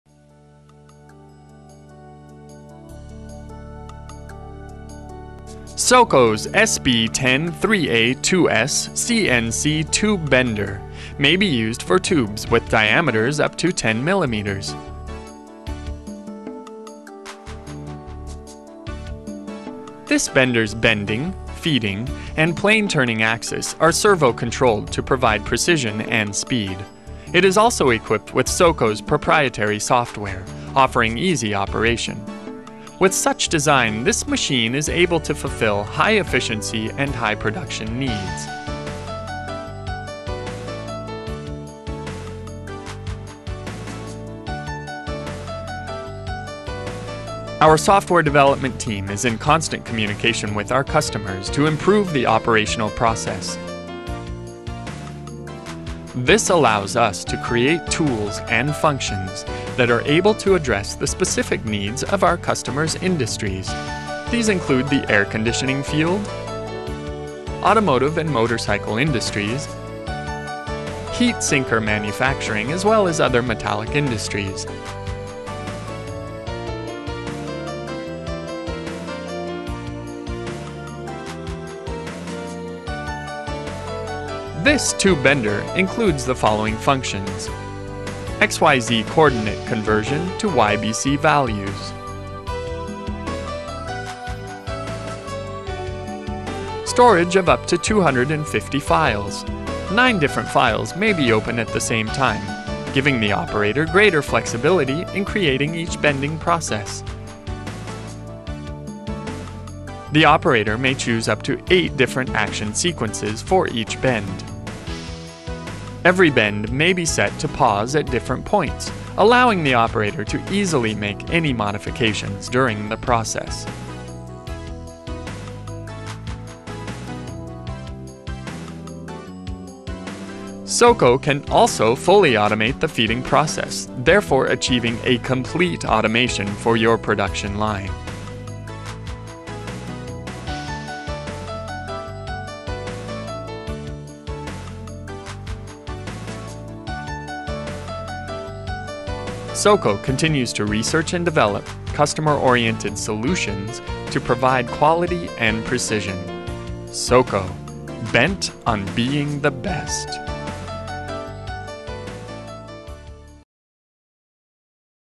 Langbow Limited, Soco SB10x3A-2S CNC Mandrel Tube Bending Machine & Auto Loader
The Soco Tube Bending Machine supplied and supported in the UK by Langbow, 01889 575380. All Pipe benders have a 5 Year parts and labour warranty.

Langbow Ltd
Director: Mark Smith